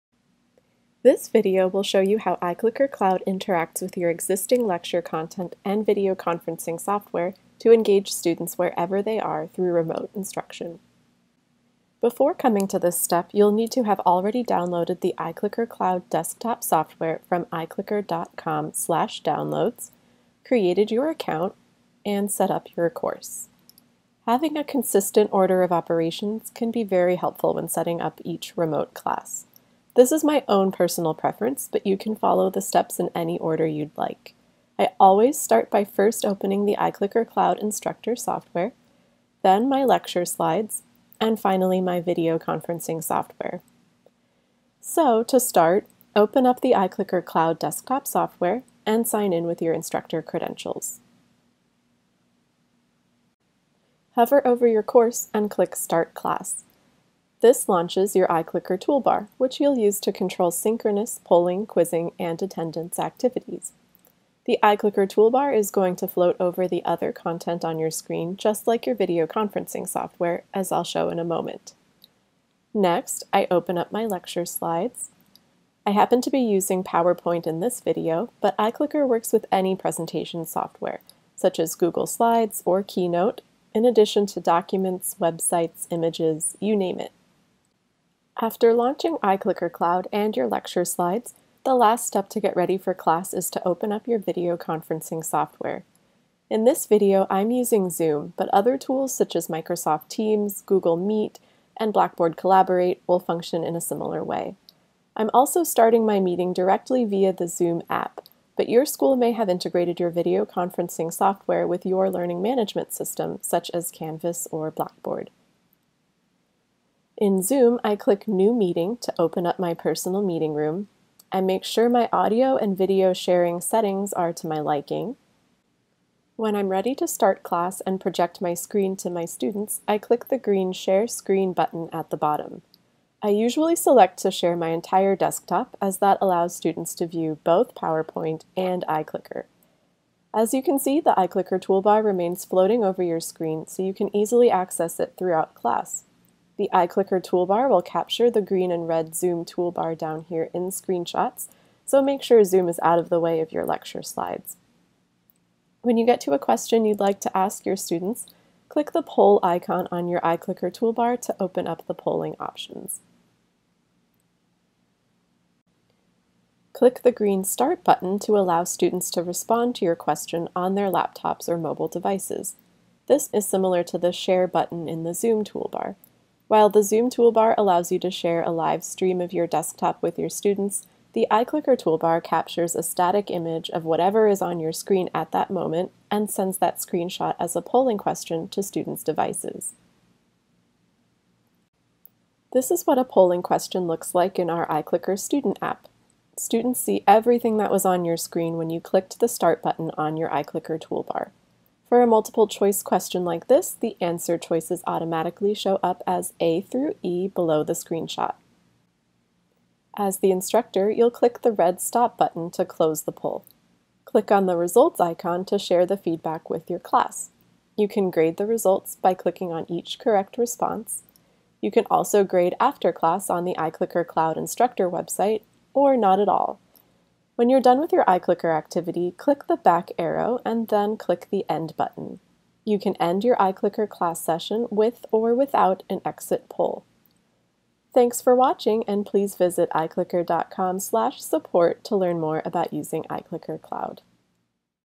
Using iClicker for Remote Instruction
This video shows how iClicker interacts with your existing lecture content and video conferencing software to engage students wherever they are.